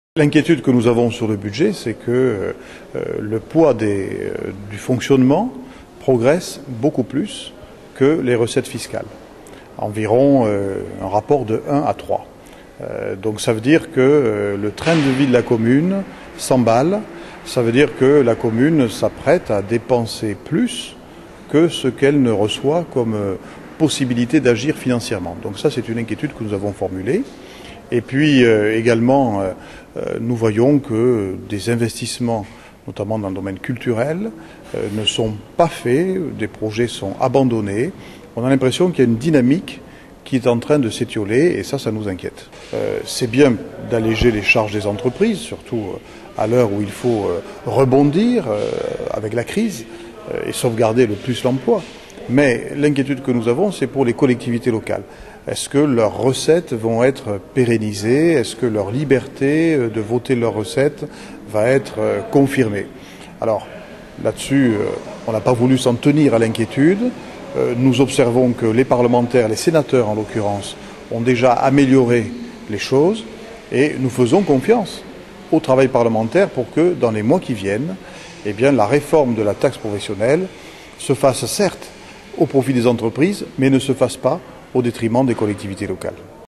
Conseil municipal de Toulouse Moudenc
Le 27 Novembre, ce sont des sujets polémiques qui ont été abordés au Conseil Municipal de Toulouse. Le budget officiel de l'année 2010 invitait forcément à un débat mouvementé à propos des choix financiers de la ville. Un sujet d'autant plus révélateur dans un contexte difficile avec la réforme des collectivités locales et la suppression de la taxe professionnelle.
Principal opposant de longue date, l'ancien maire de Toulouse Jean-Luc Moudenc a exprimé ses doutes.